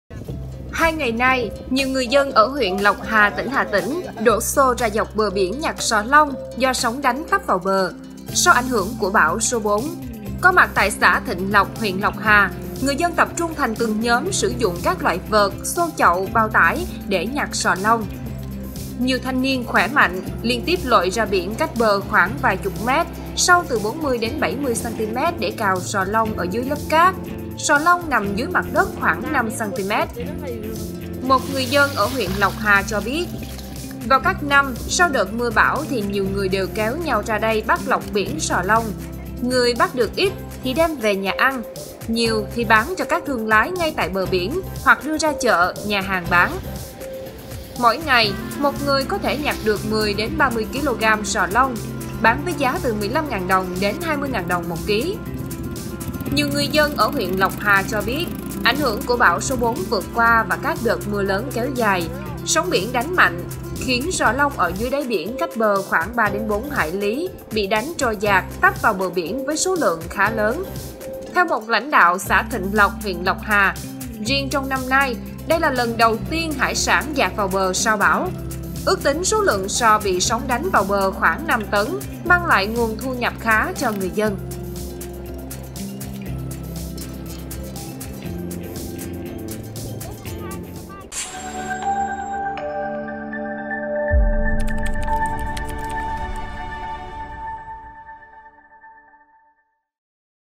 Khoảng 5 tấn sò lông dạt vào bờ, người dân Hà Tĩnh phấn khởi đi nhặt "lộc biển"
Nhiều người dân ở huyện Lộc Hà cho biết, ảnh hưởng của bão số 4 vừa qua và các đợt mưa lớn kéo dài, sóng biển đánh mạnh khiến sò lông ở dưới đáy biển cách bờ khoảng 3-4 hải lý bị đánh trôi dạt, tấp vào bờ biển với số lượng khá lớn.